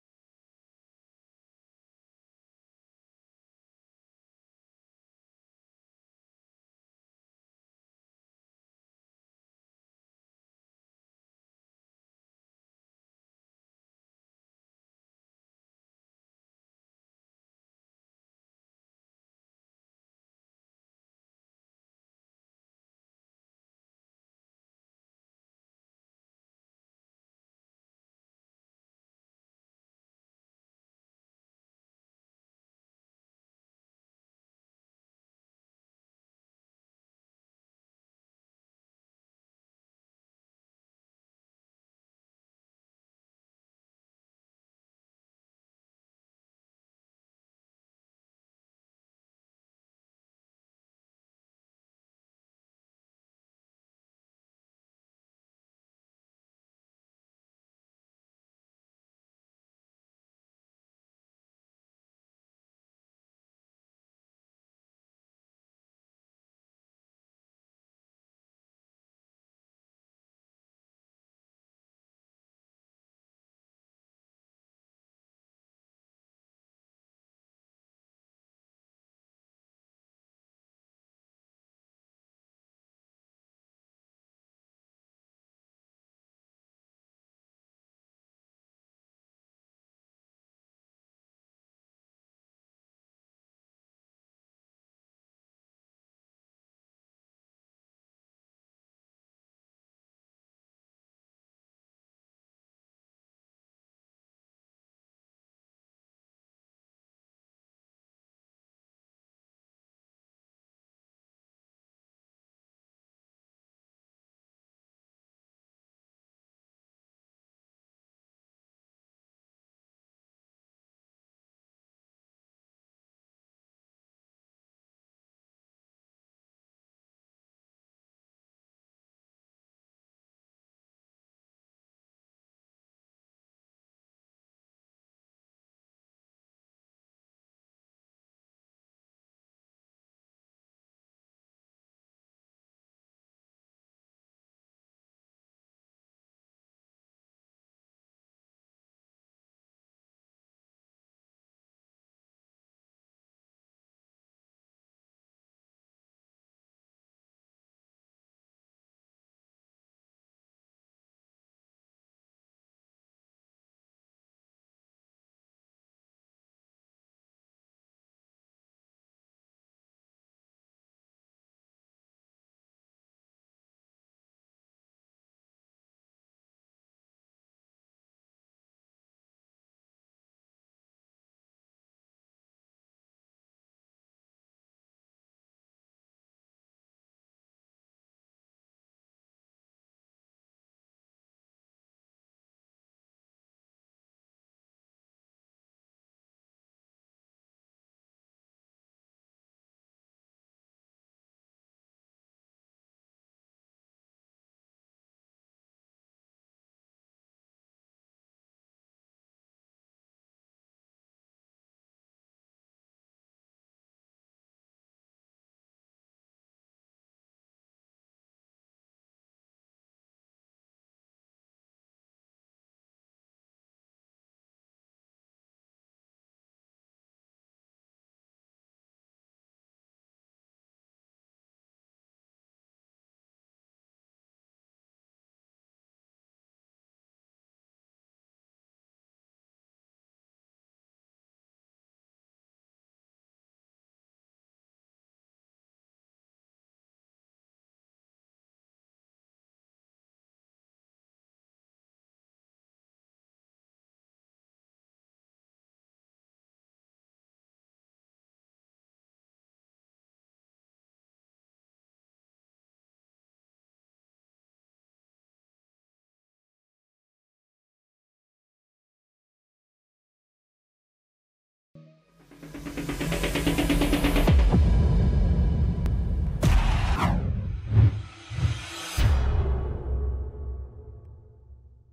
Jeremiah Watson - LB - 2/25/24 - Unedited Raw Video
NXGN Elite Regional - Cleveland Indoor filmed by SkillShow. The Spire Institute. Geneva, OH. February 25, 2024.